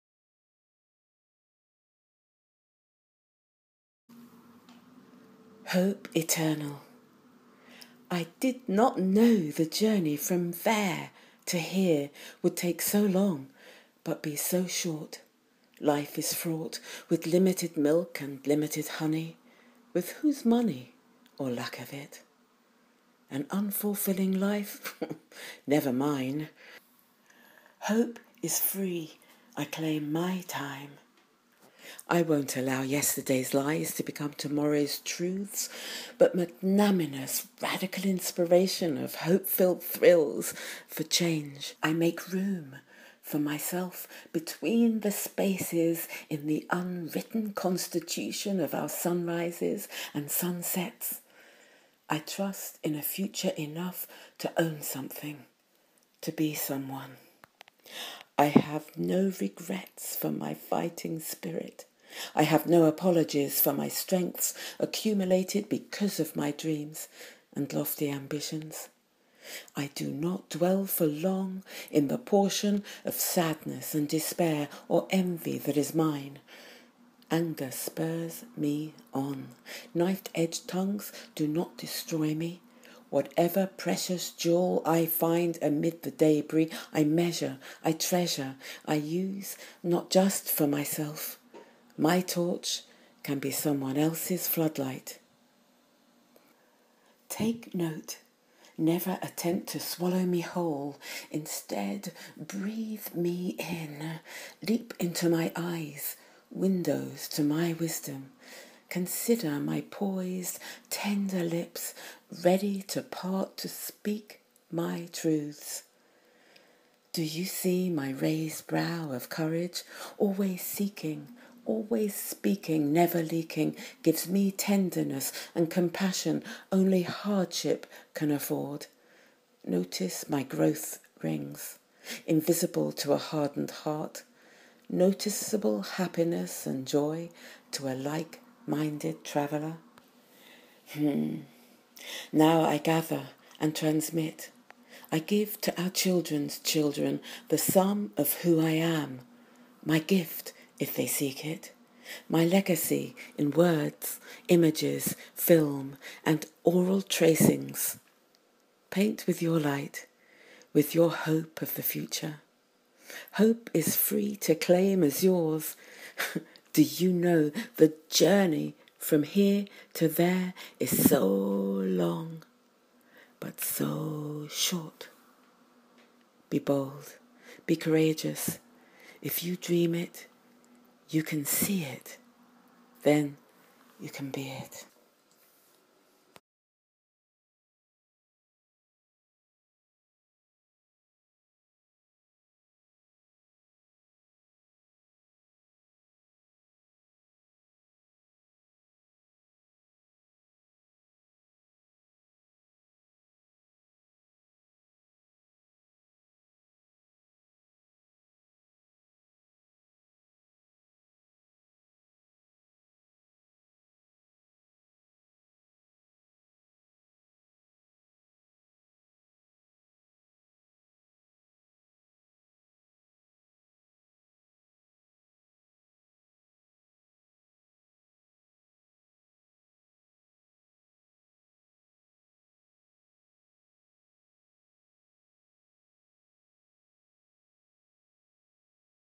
Hope Eternal (2015)
A poem by Ebun Culwin inspired by Fowokan Kelly's Another Queen Mother